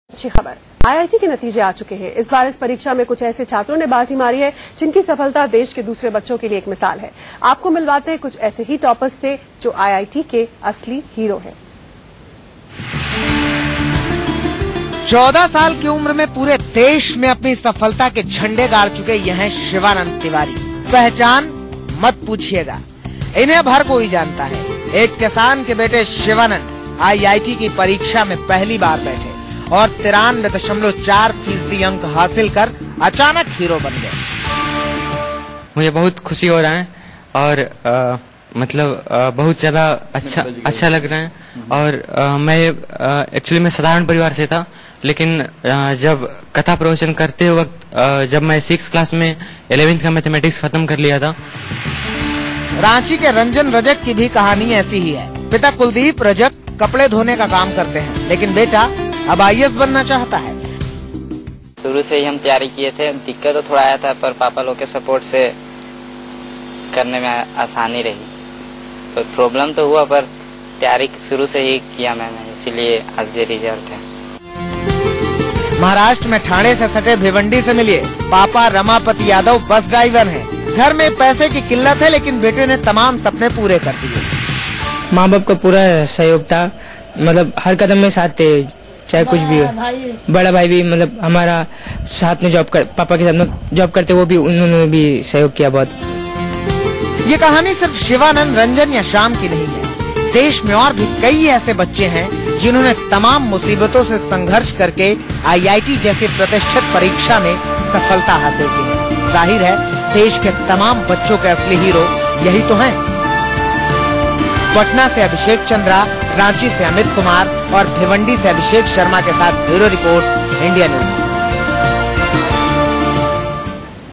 Real hero who cracked IIT
The person who cracked IIT exams have an interview with india news.